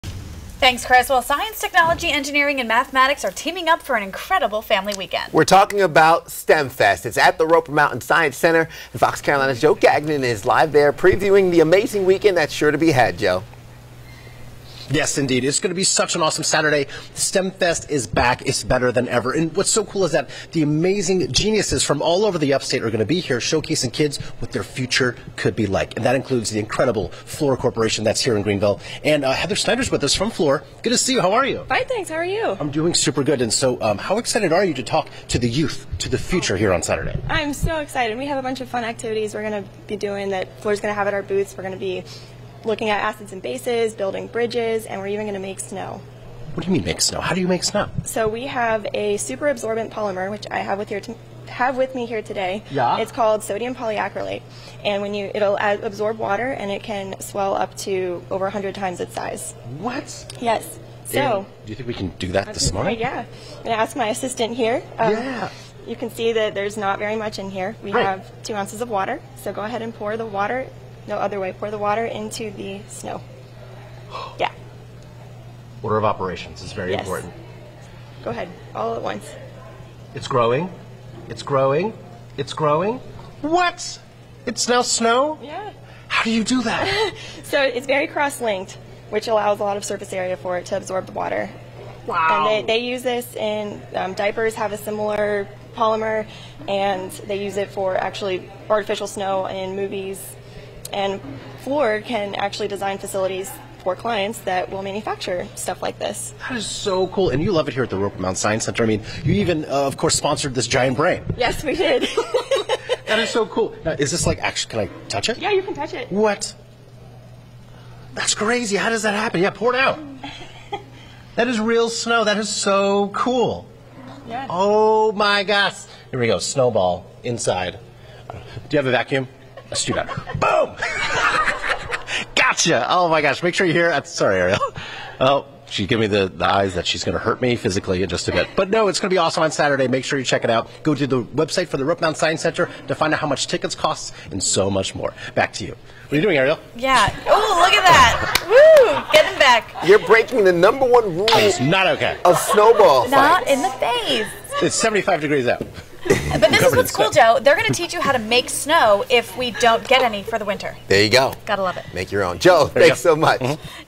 Roper Mountain Stemfest
Joe is a Roper Mountain Science Center for STEMFEST and gets pelted with some specially made snow.